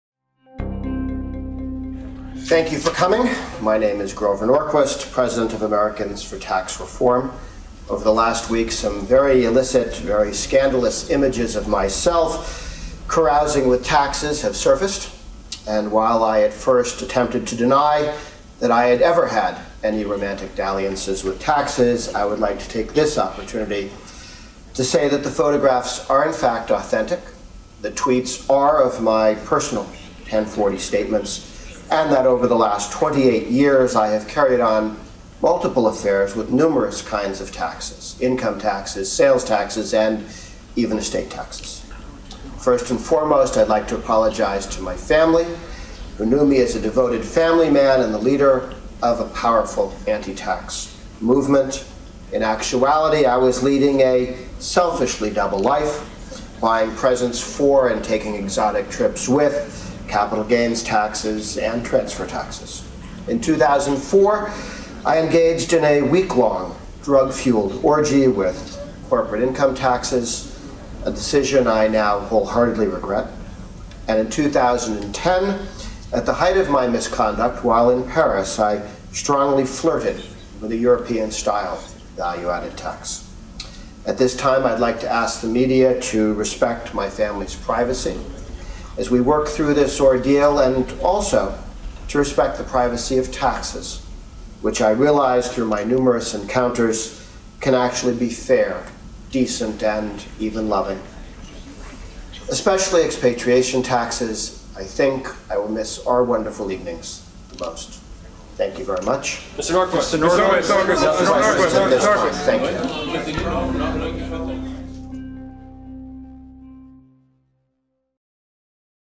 Grover Norquist: 'I Engaged In A Week-Long Drug-Fueled Orgy With Corporate Income Taxes'
President of Americans for Tax Reform Grover Norquist confirms that he carried on a 28-year salacious affair with taxes.